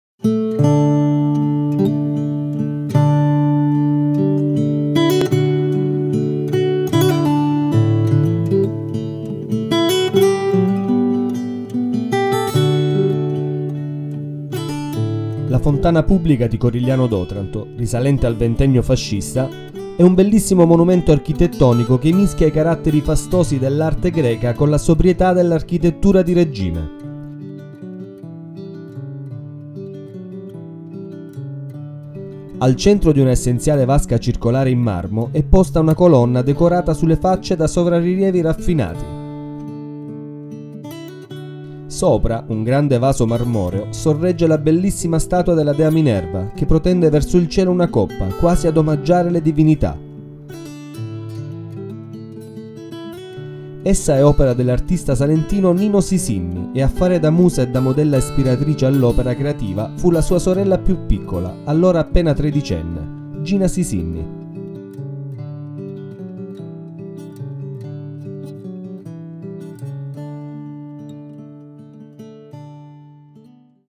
Città di Corigliano d'Otranto - Fontana pubblica
La fontana pubblica di Corigliano, risalente al ventennio fascista, è un bellissimo monumento architettonico che mischia i caratteri fastosi dell’arte greca con la sobrietà dell’architettura di regime.
Essa è situata al centro della piazza, rialzata da un gradino dal battistrada e contornata da quattro lampioni posti agli angoli.
Al centro di una essenziale vasca circolare in marmo, è posta una colonna decorata sulle facce da sopra-rilievi raffinati.